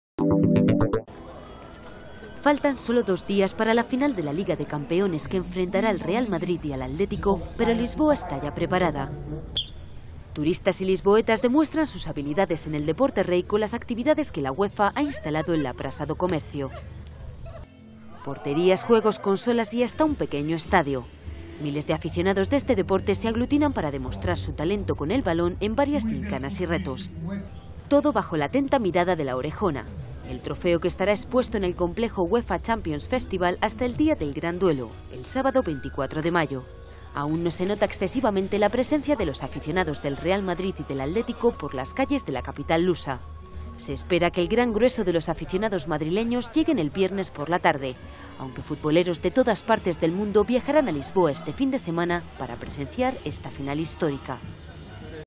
Lisboa ya está preparada para la final
Lisboa, 22 may (EFE).- (Imagen:  Adrián Espallargas)  Faltan sólo dos días para que Lisboa acoja la final de la Liga de Campeones, pero la capital lusa ya rezuma fútbol por sus principales plazas y "ruas" en las que turistas y lisboetas demuestran sus habilidades técnicas en el deporte rey.